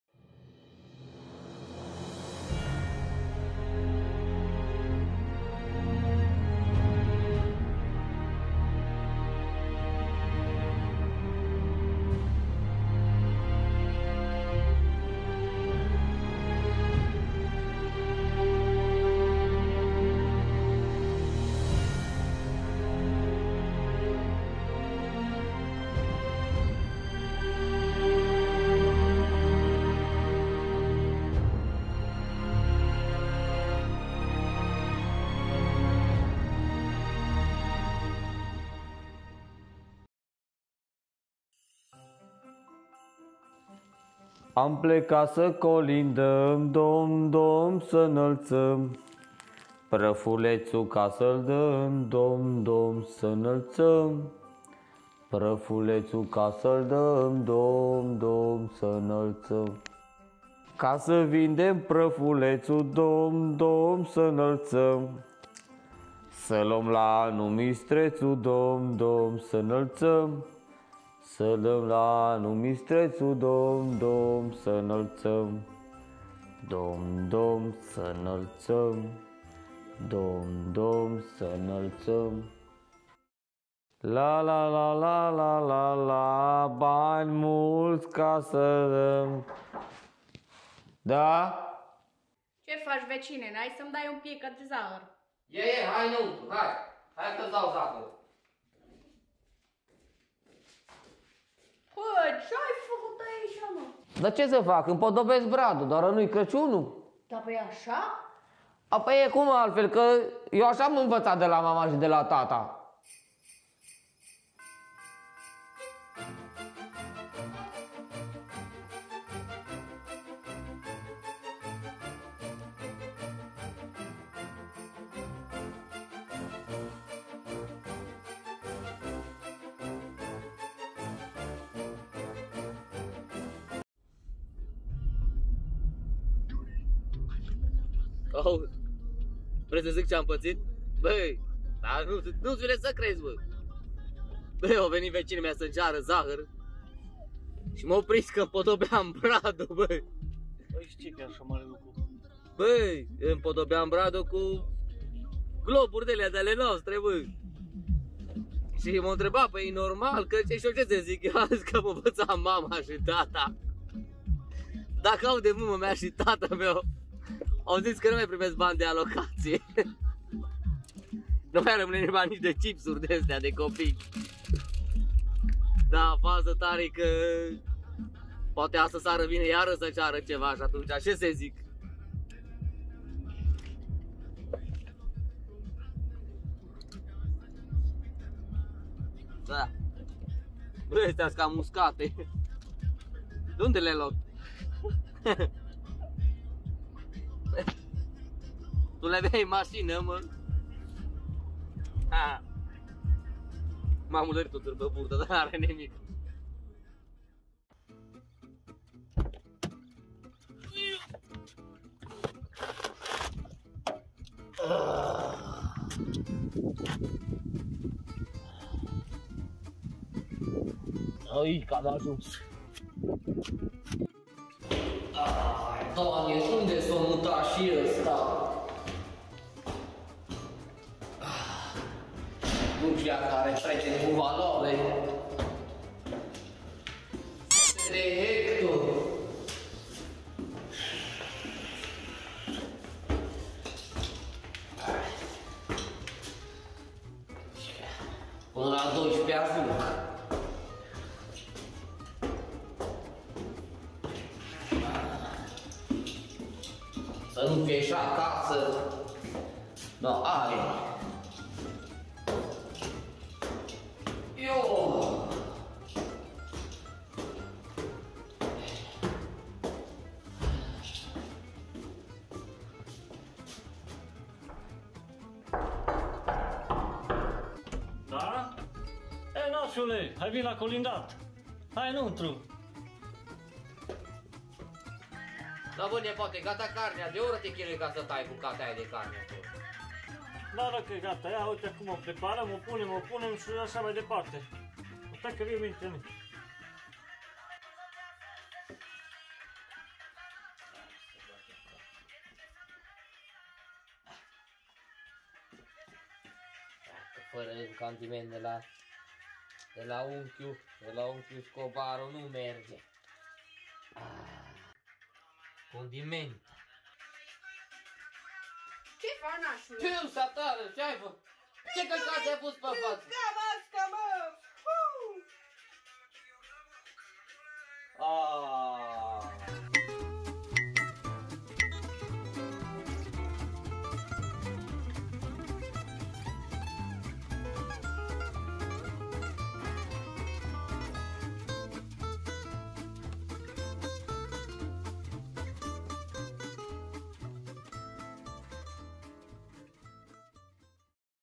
Paul Scobaru de craciun (Episod Pilot)
Inceputul noului serial pe canalul nostru gringo production despre povestea si peripetiile vestitului traficant de narcotice Paul Scobaru versiunea (Romaneasca) de pamflet.